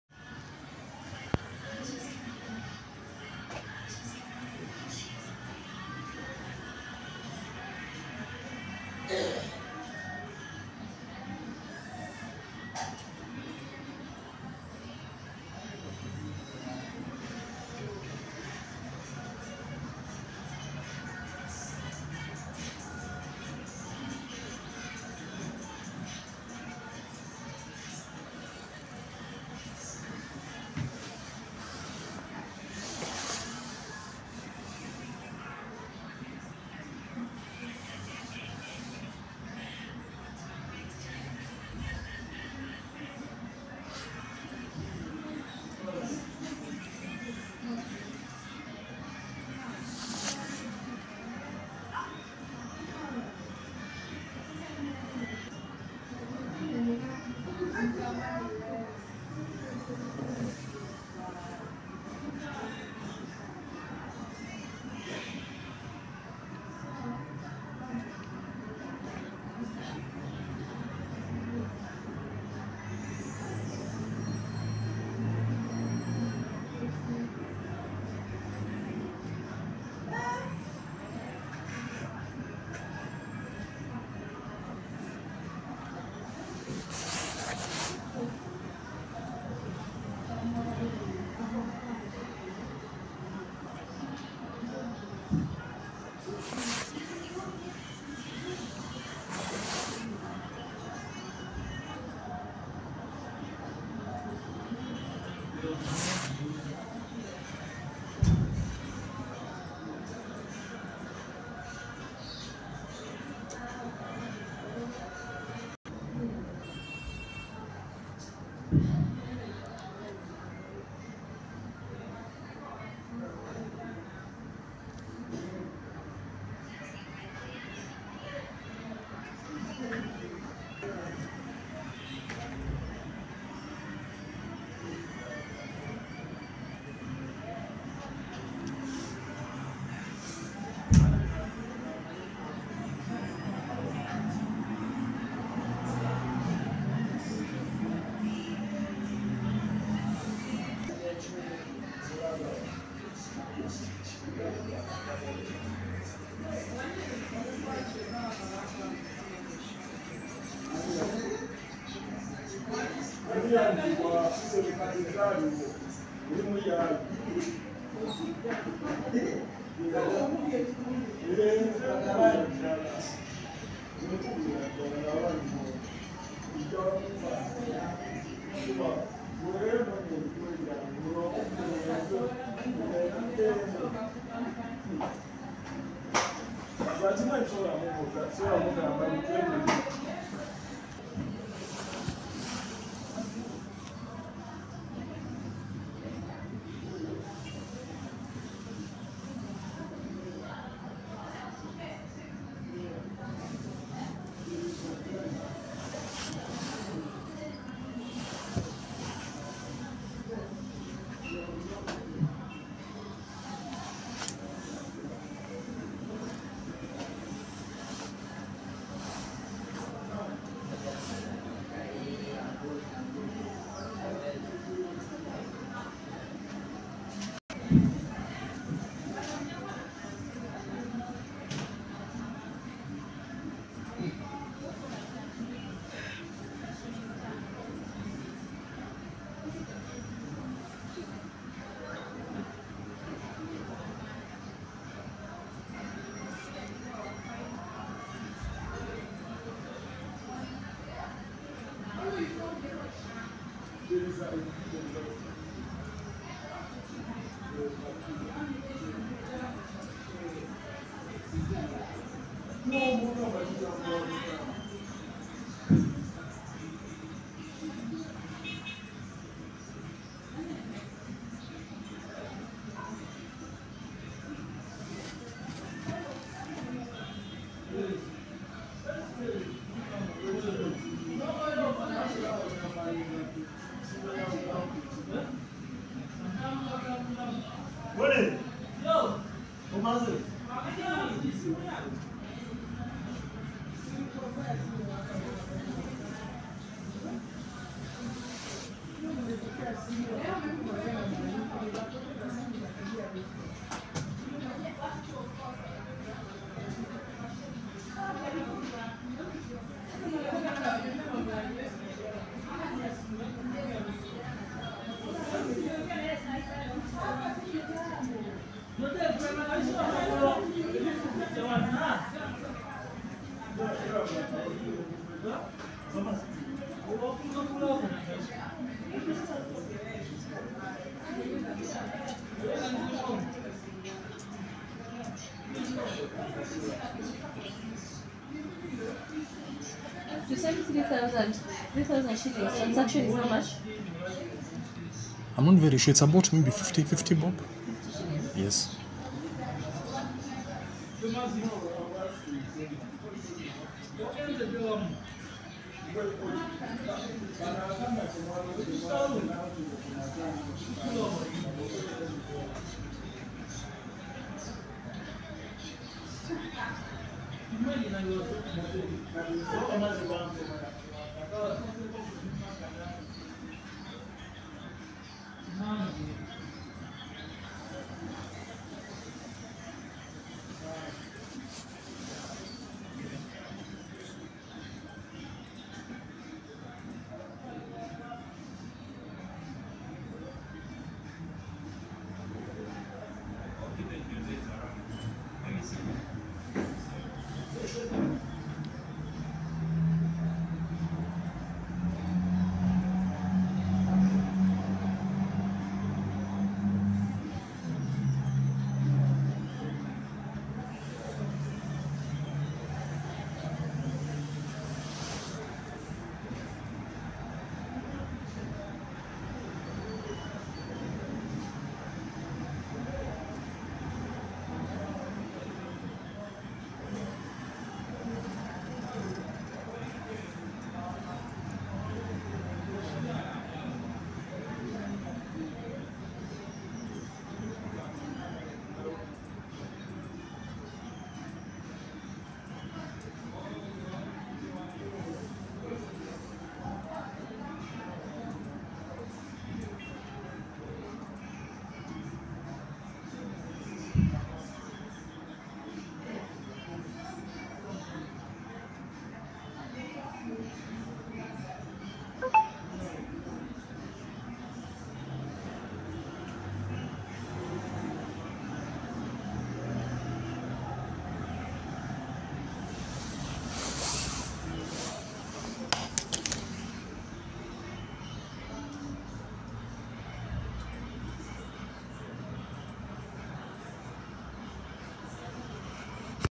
Branded Click Pens (KCPE 2014 - Social Studies) - Matthew Karani (FB Staff/ Arrow Medical, K Street)
StanChart/ NSSF/ Makini School - Data Scientist

QN 49 - Daniel Moi's greatest contribution

QN 53 - Shipping of Medicine

QN 59 - Highest Court in Kenya (Court of Apple - Steve Jobs)

QN 22 - Mining of minerals

Matthew received his KCPE results while on Christmas vacation at Digula, Sabatia